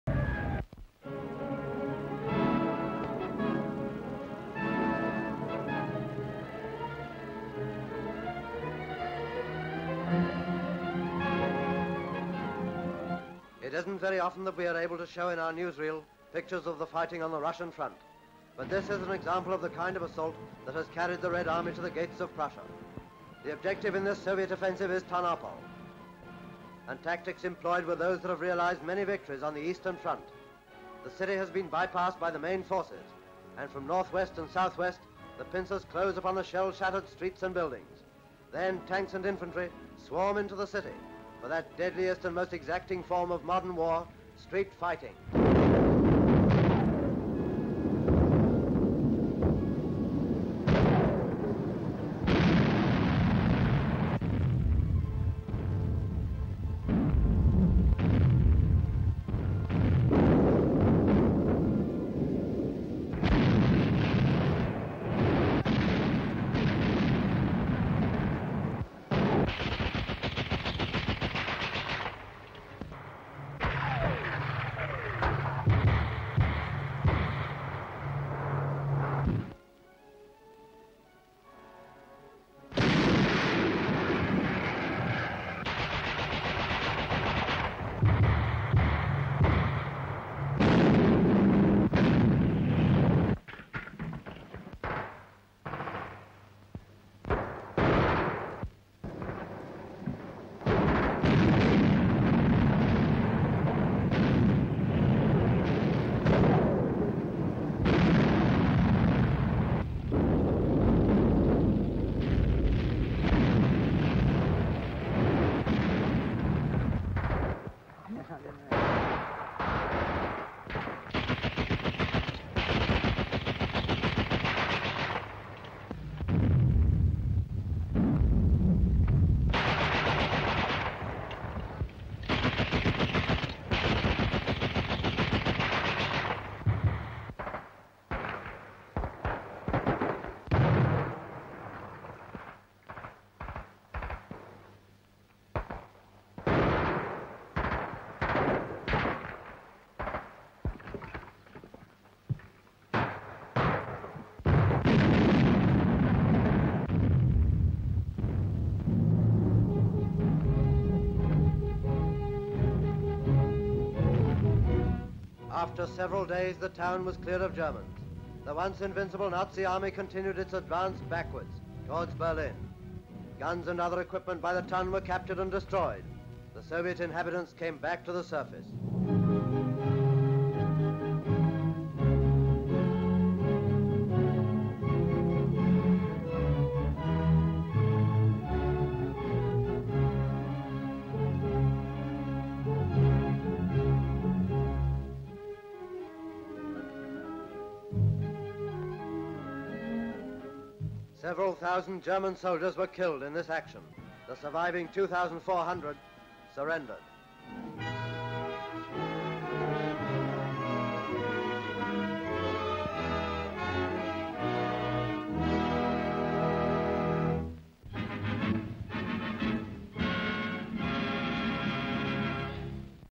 Soviet troops battle in Russia (1944)
Soviet Army advances on the Eastern Front against Wehrmacht units in WWII

Full Description:

SLATE INFORMATION: The War in Russia: Savage Battles Liberate Another Soviet Town

RUSSIA: Various:
EXT

PRISONERS OF WAR. Germans taken in Tarnopol, Russia

RUSSIA. Troops recapture TARNAPOL..street fighting..& German prisoners

STREET FIGHTING. In Tarnapol

Soviet Union; Military - Active

Background: Soviet Army advances on the Eastern Front against Wehrmacht units in WWII